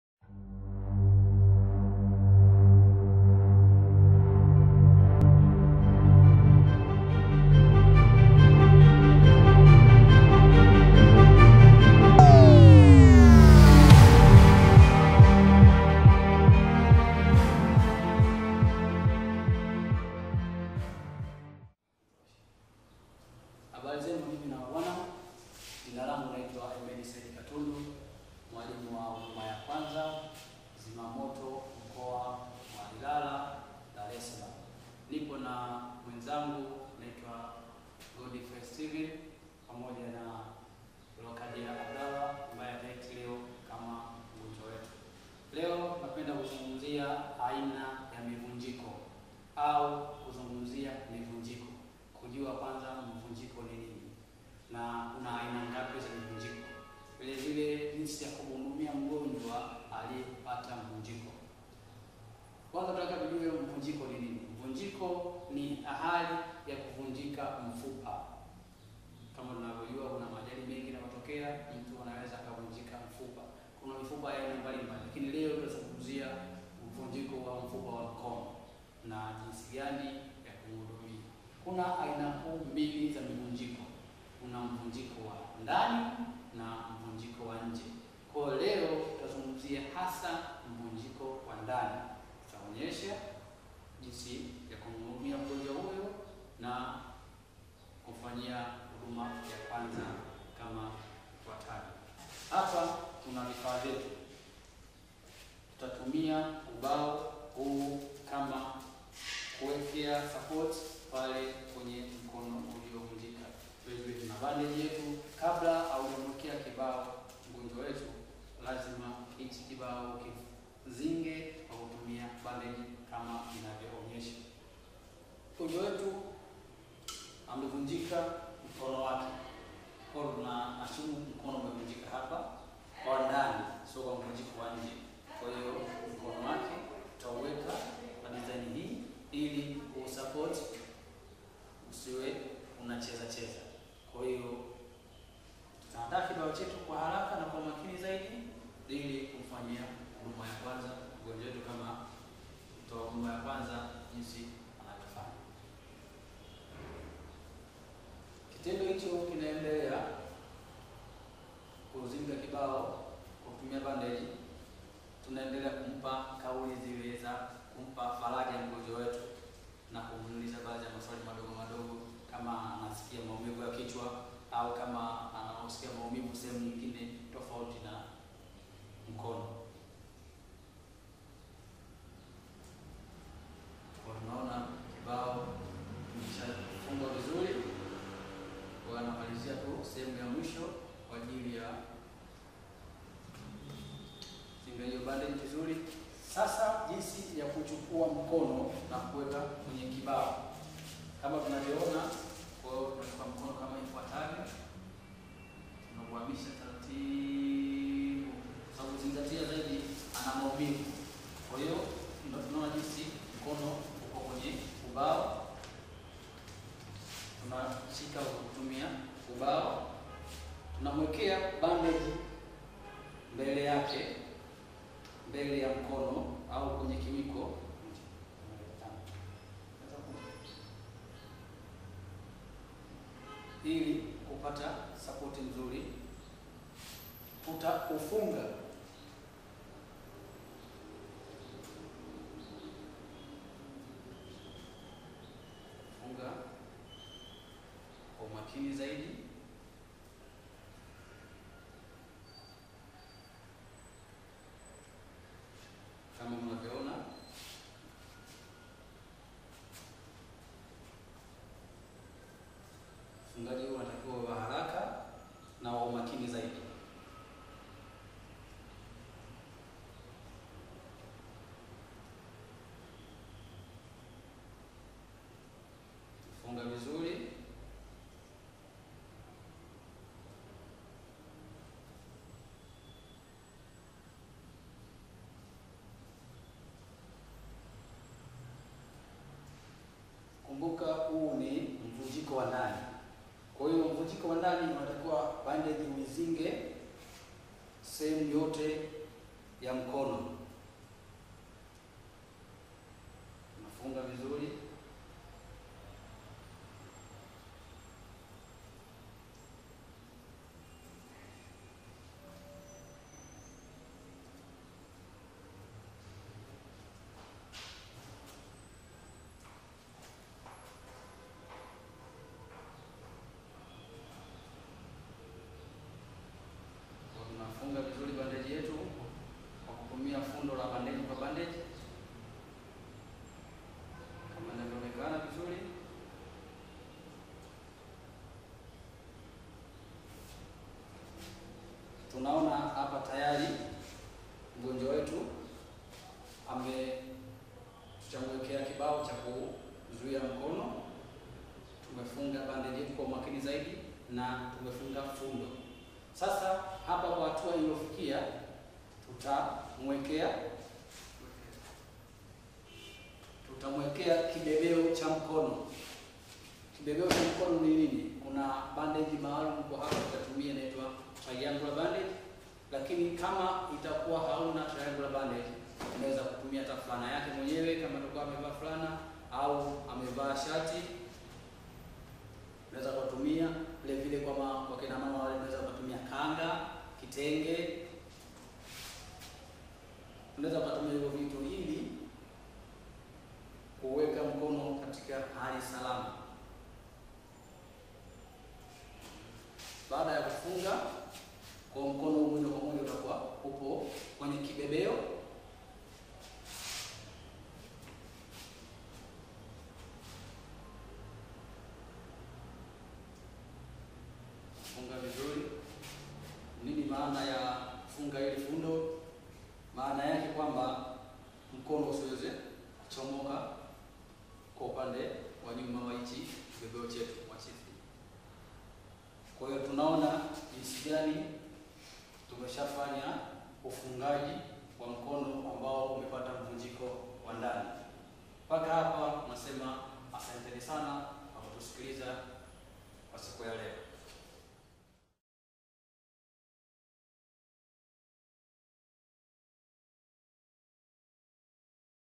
Huduma ya kwanza - Splint
First Aid instruction videos produced by the Fire and Rescue Force Dar es Salaam.